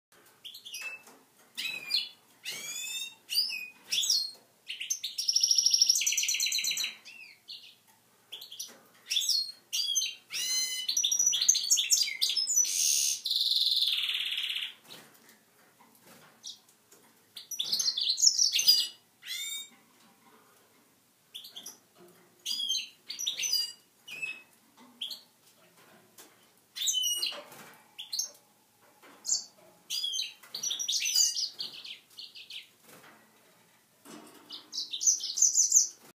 GOLDFINCH MULE SINGING TO THE CANARY HENS
my mule, JACK singing away, trying to catch the attention of the canary hens!